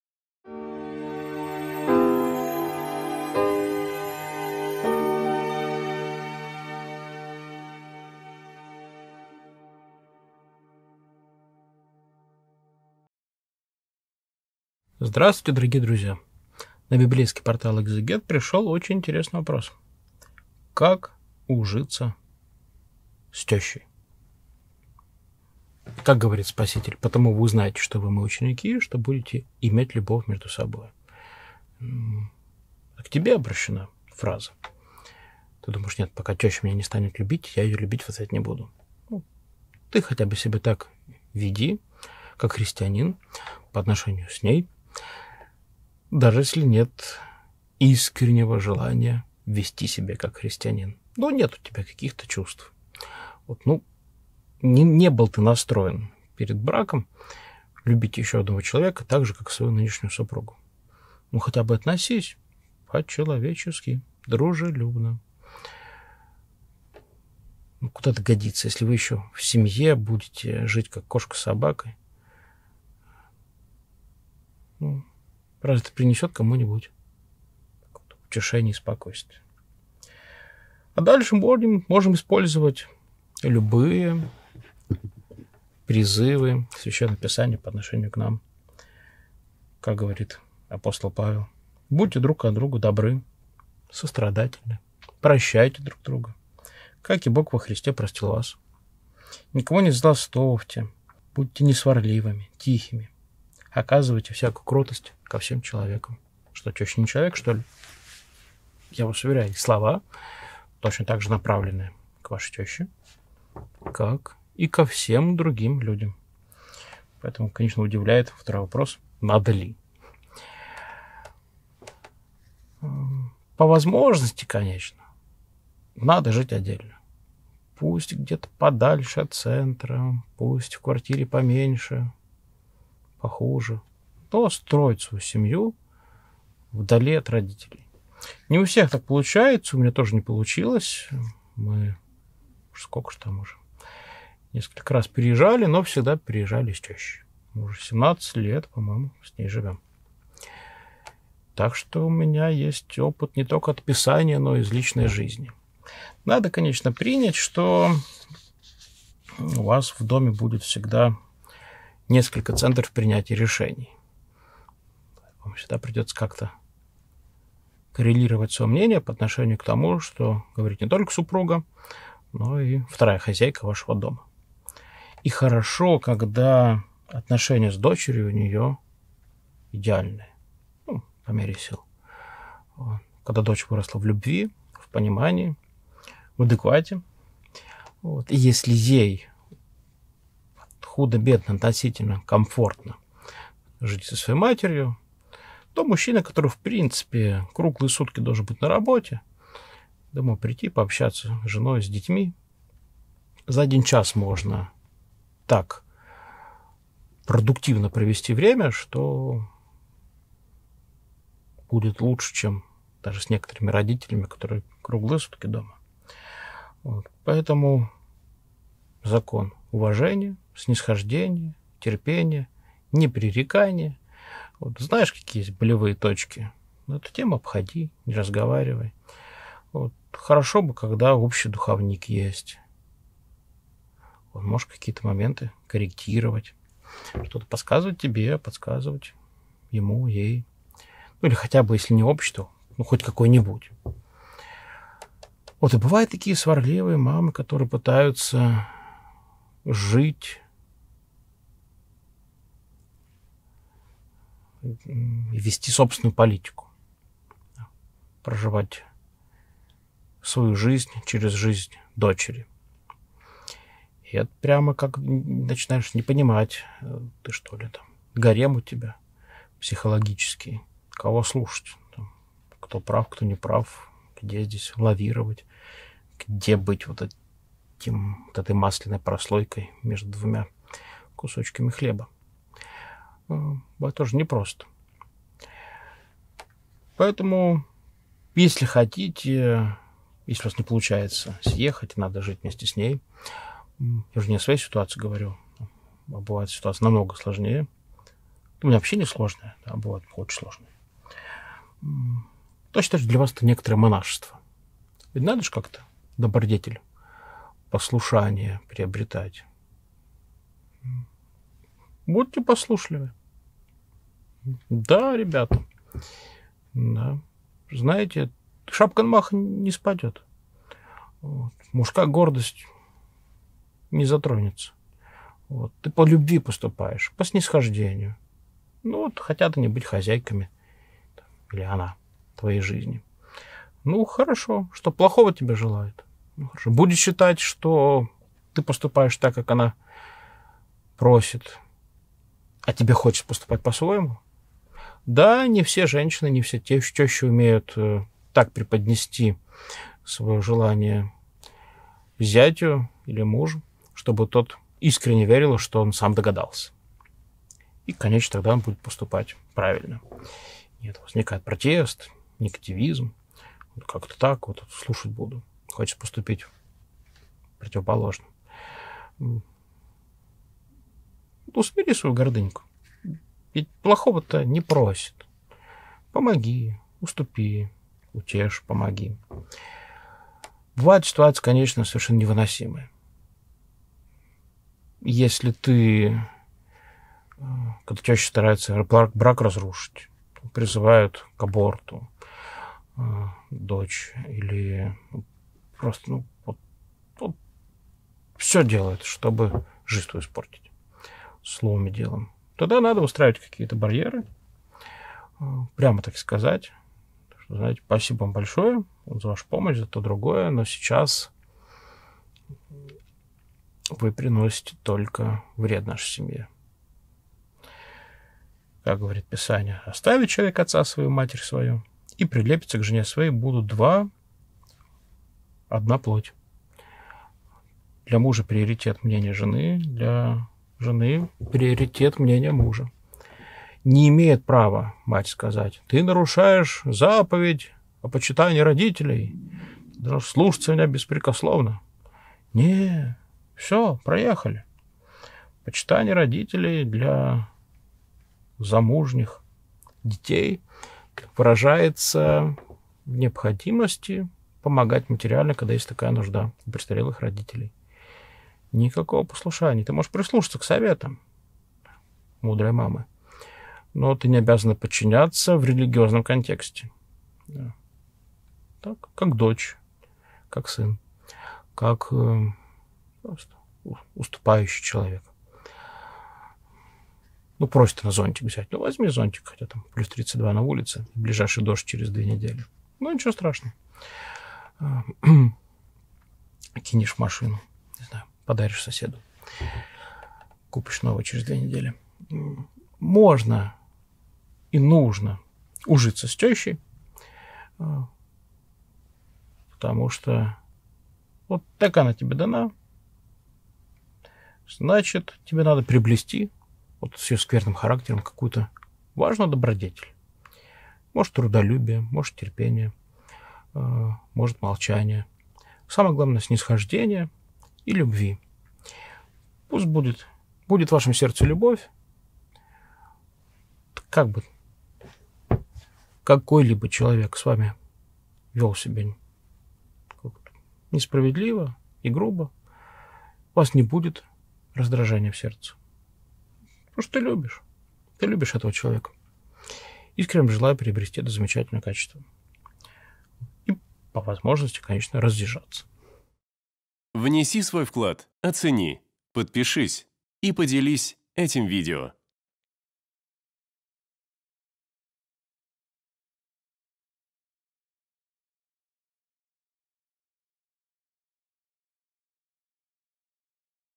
КАК УЖИТЬСЯ С ТЁЩЕЙ ? Священник Владислав Береговой
Команда Экзегет.

Библейский портале Экзегет вместе со священником Владиславом Береговым отвечает на вопрос: "Как ужиться с тёщей?"

🔸   Дружелюбие
🔸   Взаимоотношения в семье
🔸   Уважение и терпение
🔸   Совет духовника
🔸   Послушание
🔸   Поступки по любви
🔸   Ужиться можно и нужно

📜 Читать Послание к Ефесянам главу 4, стих 32 (с толкованиями)